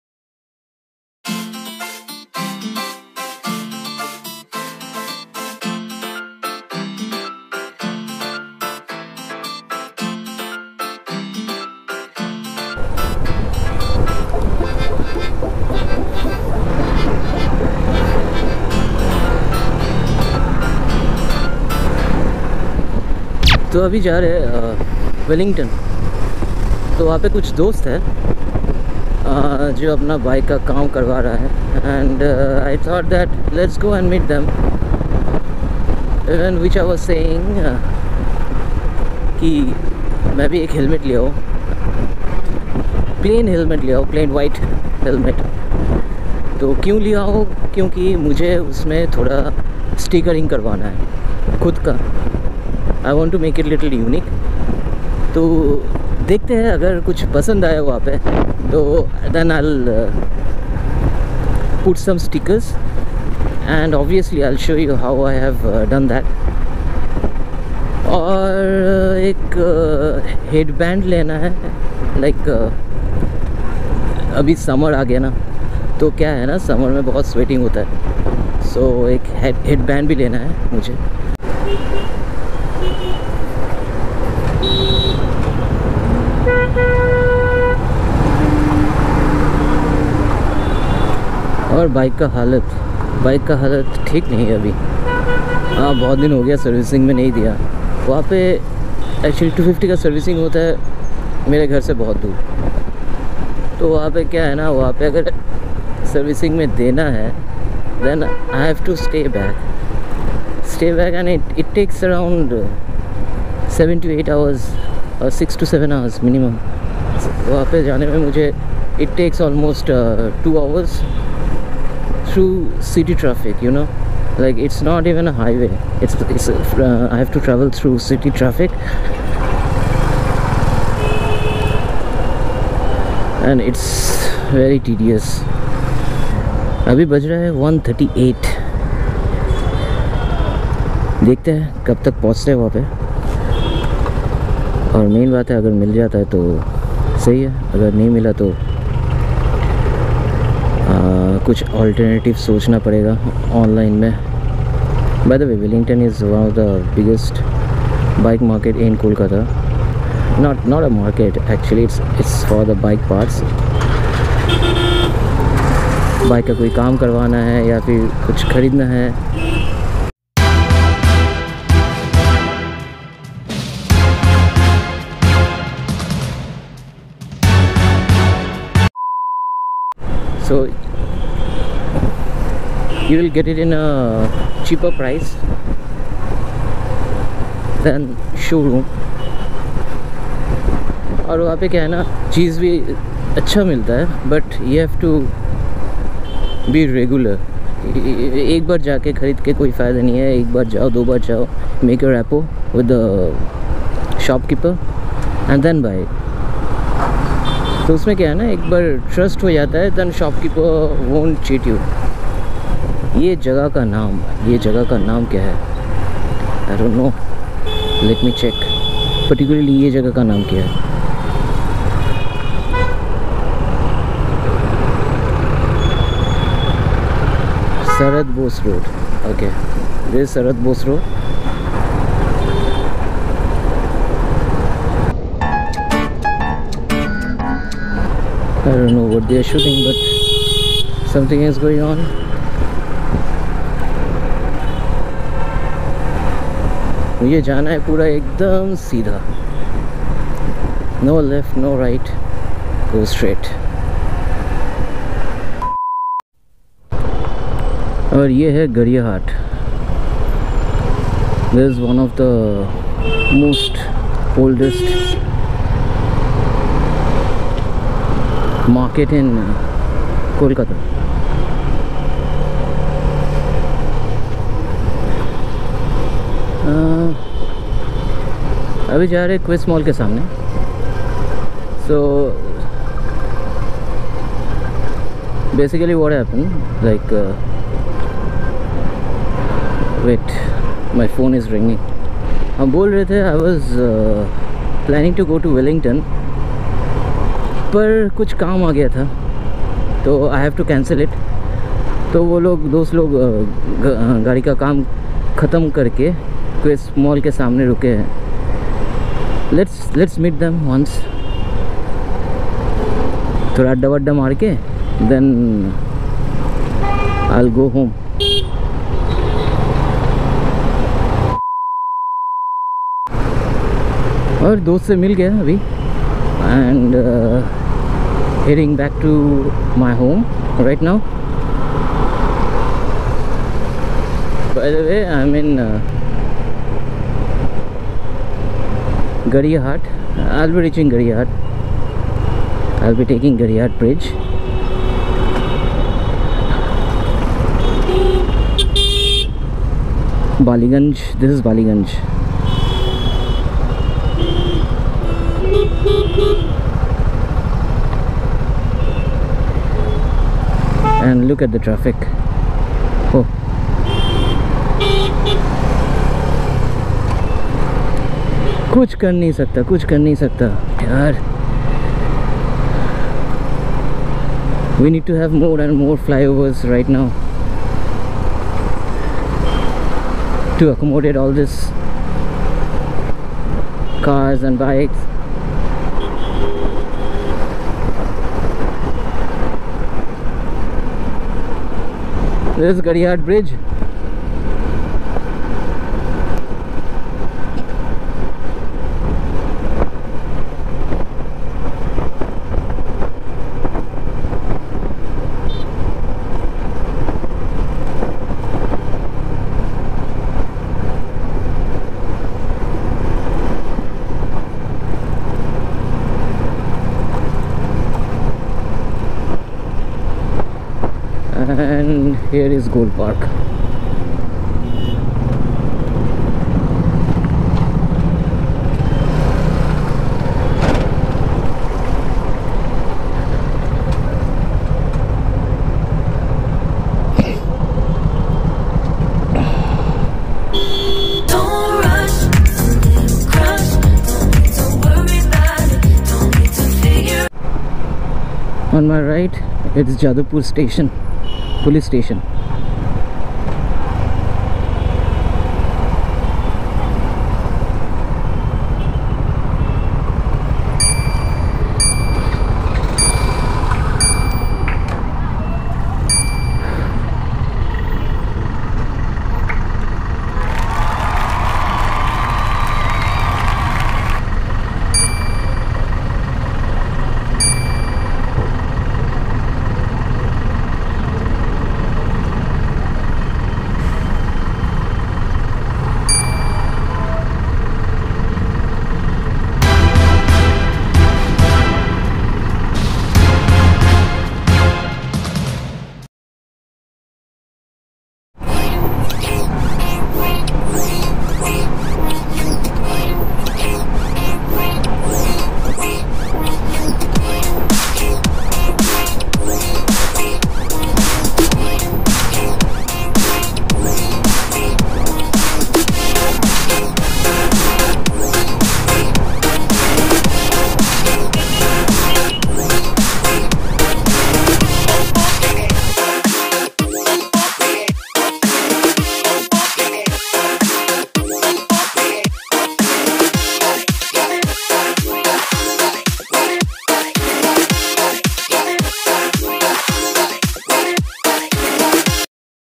HAD AN ENCOUNTER WITH THE DON OF KOLKATA ||Exploring Kolkata road condition || Traffic || Gixxer 250
In this video I was exploring kolkata and tried to show the traffic condition in Kolkata thru my lens ..  with subtitles

Bike : Gixxer 250
Lens : Gopro 5